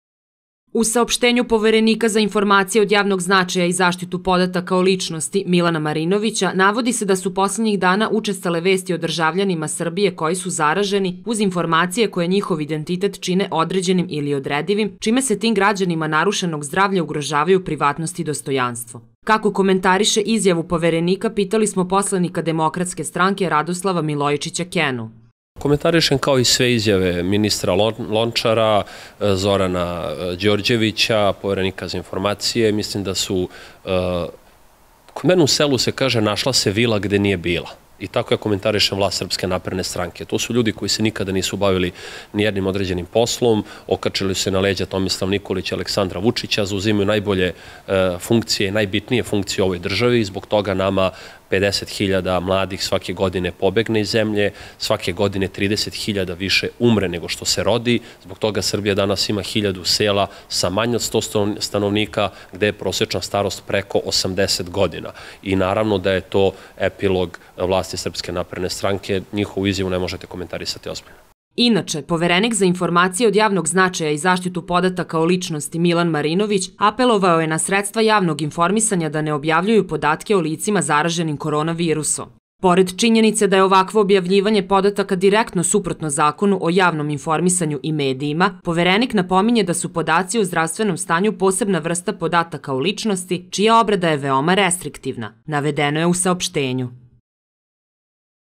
Radoslav Milojičić Kena: Partokratija Srpske napredne stranke pustoši Srbiju
Radoslav Milojičić Kena je izjavio da partokratija Srpske napredne stranke pustoši Srbiju, te da zbog toga ne može ozbiljno da komentariše nijednu izjavu funkcionera kojeg je  postavila ta stranka.